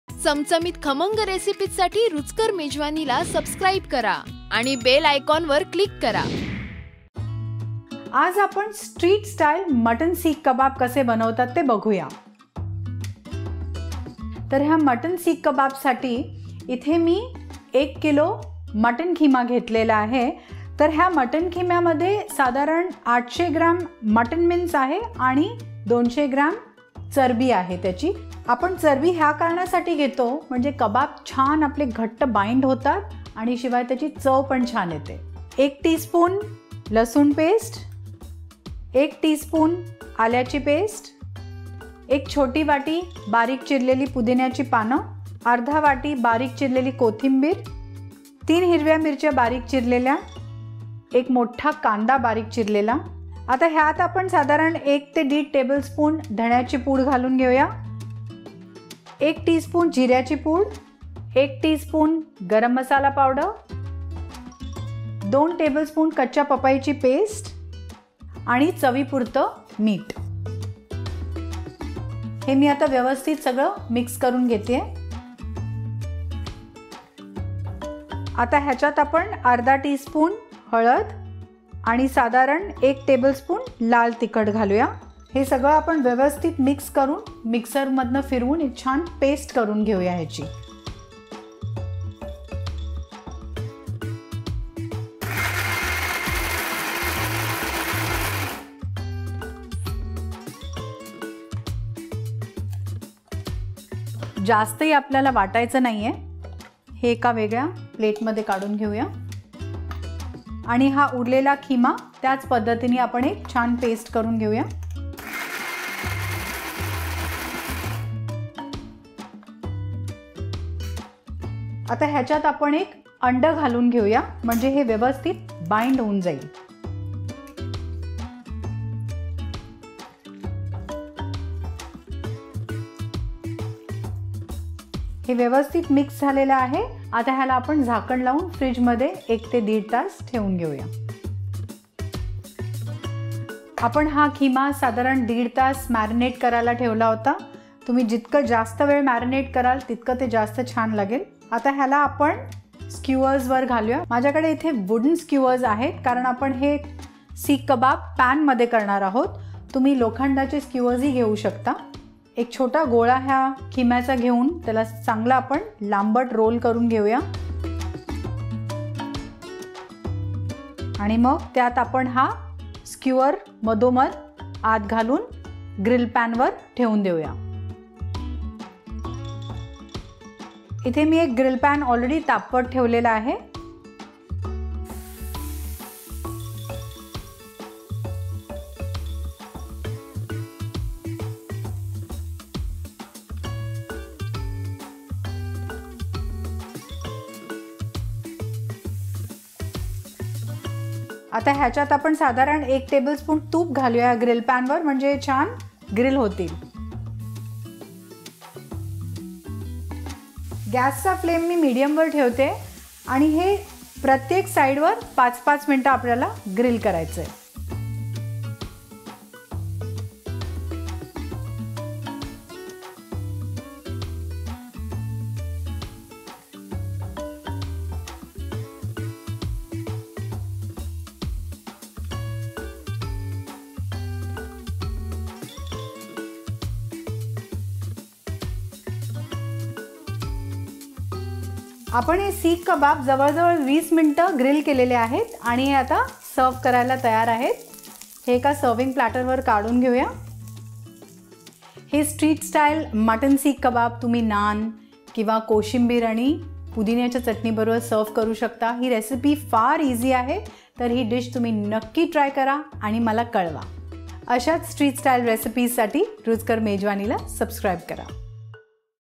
मटण सीख कबाब - Mutton Seekh Kebab | Street Style Mutton Seekh kabab | Mutton Kabab Recipe - Smita
Learn how to make Street Style 'मटण सीख कबाब" - Mutton Seekh Kabab' with our chef Smita Deo.

Chicken Seekh Kabab is made from minced chicken, spices and can be found at most of the dhabas and on the streets. It is grilled over a charcoal fire, but it can also be made at home using a grill pan.

Ingredients:
(Serves 2)
- 1 Kg Mutton(minced)
- 1 tsp Garlic Paste
- 1 tsp Ginger Paste
- 1/4 cup Mint Leaves
- 1/2 cup Coriander Leaves
- 3 Green Chillies(chopped)
- 1 Onion(chopped)
- 1 1/2 tbsp Coriander Seeds Powder
- 1 tsp Cumin Seeds Powder
- 1 tsp Garam Masala
- 2 tbsp Raw Papaya paste
- Salt
- 1/2 tsp turmeric Powder
- 1 tbsp Red Chilli Powder
- 1 Egg
- 1 tbsp ghee

Method:
1. Take minced mutton in a bowl and add garlic paste, ginger paste, mint leaves, coriander leaves, chopped green chillies, chopped onion, coriander seeds powder, cumin seeds powder,garam masala, raw papaya paste, salt
2. Mix all of it together, and then, add turmeric powder and red chilli powder
3. Now, grind this mixture into a thin paste and take it out in a plate
4. After this, crack one egg on the paste; mix it, cover it and refrigerate it for around 1 - 1 1/2 hour
5. Now, take some mutton paste and give it a cylindrical shape
6. Put the wooden skewer right through the bottom of the paste, to its top and place it over a grill pan
7. Add ghee over the grill pan; grill it on both the sides for 5 minutes each and keep the flame on medium
8. Grill these seekh kababs for around 20 minutes and then take it out on a serving platter

If you like this recipe 'मटण सीख कबाब - Mutton Seekh Kabab', you can also try other such Mutton recipes like:

Host: Smita Deo
Copyrights: REPL